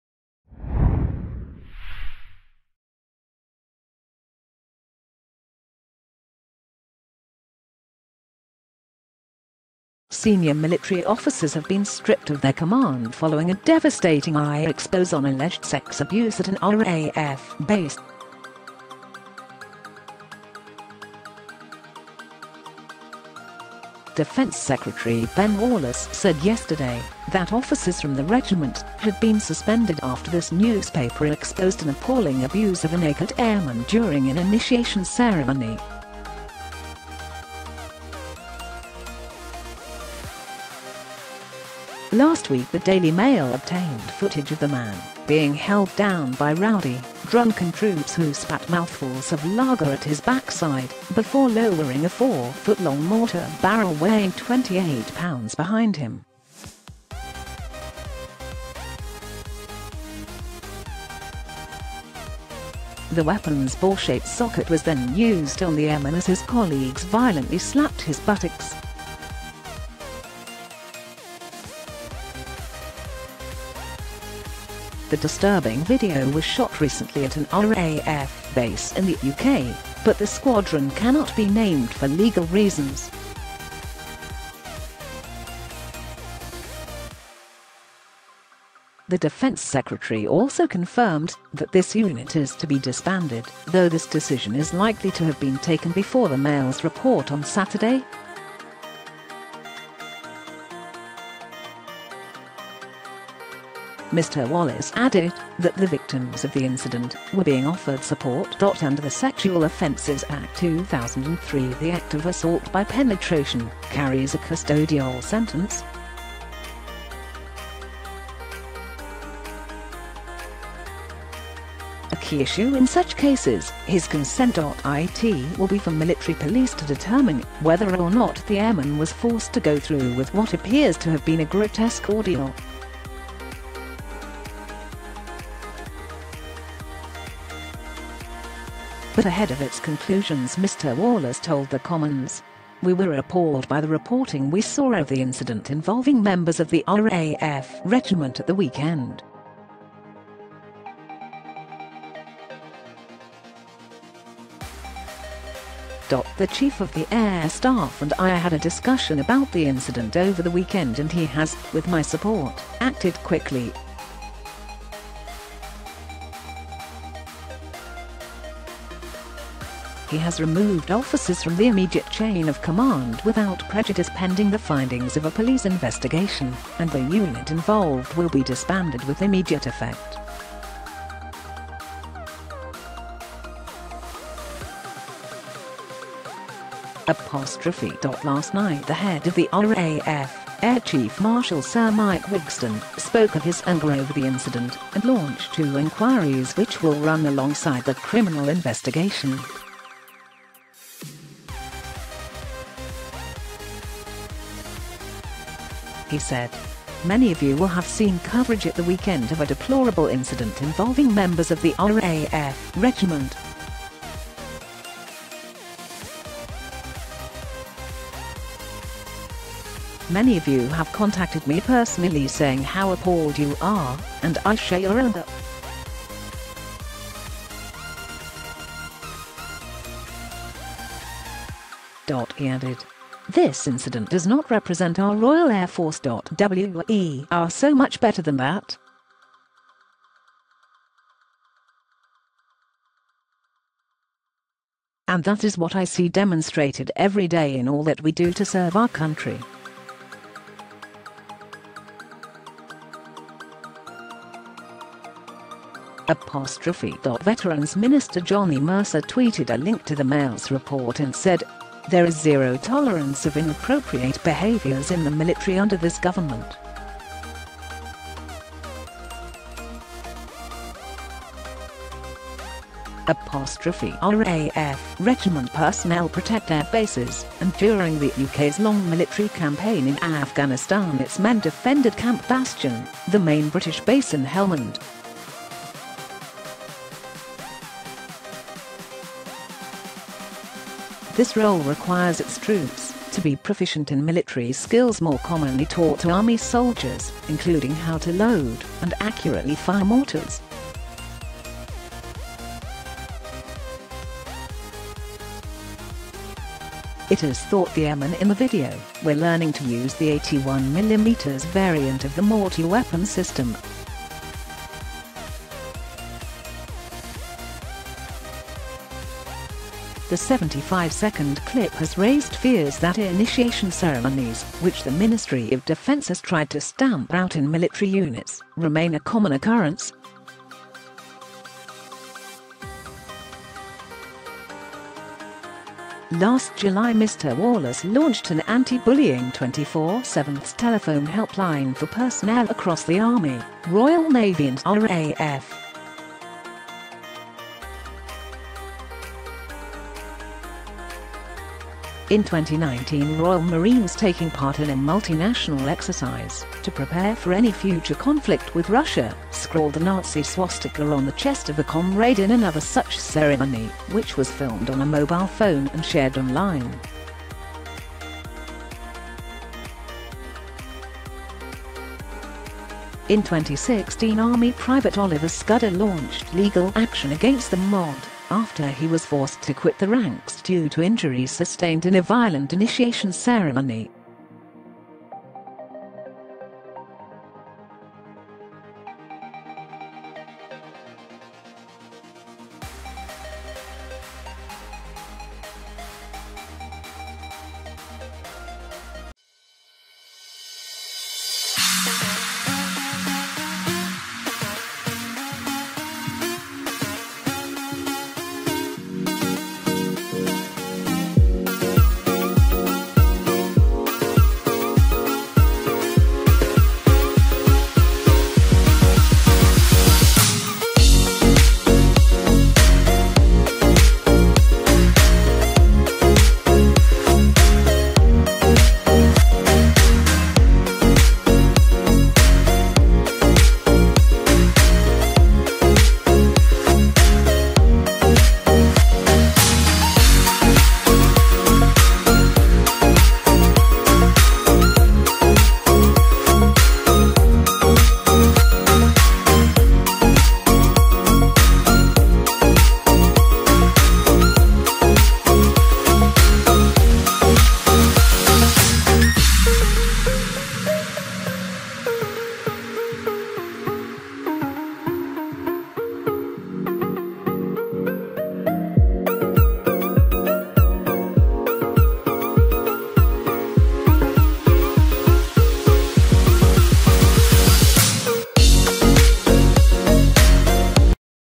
Officers get stripped of command following vile RAF initiation
Senior military officers have been stripped of their command following a devastating I exposé on alleged sex abuse at an RAF base.
Defence Secretary Ben Wallace said yesterday that officers from the regiment had been suspended after this newspaper exposed an appalling abuse of a naked airman during an initiation ceremony.
Last week the Daily Mail obtained footage of the man being held down by rowdy, drunken troops who spat mouthfuls of lager at his backside before lowering a four foot-long mortar barrel weighing 28lb behind him.
The weapon’s ball-shaped socket was then used on the airman as his colleagues violently slapped his buttocks.The disturbing video was shot recently at an RAF base in the UK but the squadron cannot be named for legal reasons.
The Defence Secretary also confirmed that this unit is to be disbanded – though this decision is likely to have been taken before the Mail’s report on Saturday.Mr Wallace added that the ‘victims’ of the incident were being offered support.Under the Sexual Offences Act 2003 the act of ‘assault by penetration’ carries a custodial sentence.
A key issue in such cases is consent.It will be for military police to determine whether or not the airman was forced to go through with what appears to have been a grotesque ordeal.
But ahead of its conclusions Mr Wallace told the Commons: ‘We were appalled by the reporting we saw of the incident involving members of the RAF Regiment at the weekend.‘
The Chief of the Air Staff and I had a discussion about the incident over the weekend and he has, with my support, acted quickly.He has removed officers from the immediate chain of command without prejudice pending the findings of a police investigation and the unit involved will be disbanded with immediate effect.’
Last night the head of the RAF, Air Chief Marshal Sir Mike Wigston, spoke of his anger over the incident and launched two inquiries which will run alongside the criminal investigation.
He said: ‘Many of you will have seen coverage at the weekend of a deplorable incident involving members of the RAF Regiment.Many of you have contacted me personally saying how appalled you are and I share your anger.’
 He added: ‘This incident does not represent our Royal Air Force.We are so much better than that.And that is what I see demonstrated every day in all that we do to serve our country.’
Veterans minister Johnny Mercer tweeted a link to the Mail’s report and said: ‘There is zero tolerance of inappropriate behaviours in the military under this Government.’ RAF Regiment personnel protect air bases and during the UK’s long military campaign in Afghanistan its men defended Camp Bastion, the main British base in Helmand.
This role requires its troops to be proficient in military skills more commonly taught to army soldiers, including how to load and accurately fire mortars.It is thought the airmen in the video were learning to use the 81mm variant of the mortar weapon system.
The 75-second clip has raised fears that ini